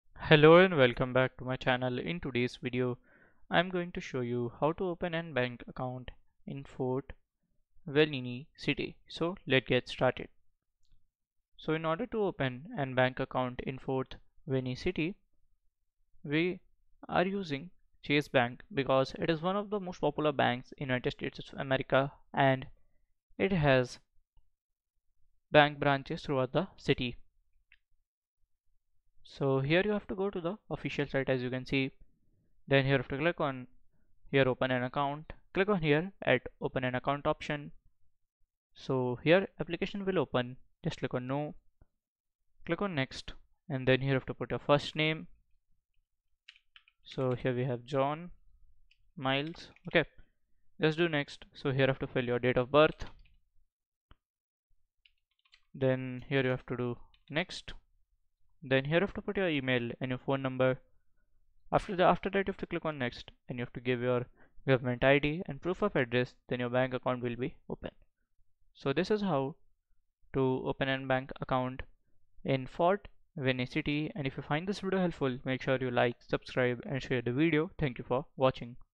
✅ How to Open A Bank Account in Fort Wayne (Full Guide)
Considering opening a bank account in the vibrant city of Fort Wayne? This comprehensive guide takes you through the entire process. From selecting the right bank to understanding the necessary documentation, we've got you covered. Tune in now and start your financial journey in the Summit City!

Key Learning Points:

Step-by-step guide to opening a bank account in Fort Wayne
Choosing the best bank for your needs
Understanding the required paperwork and identification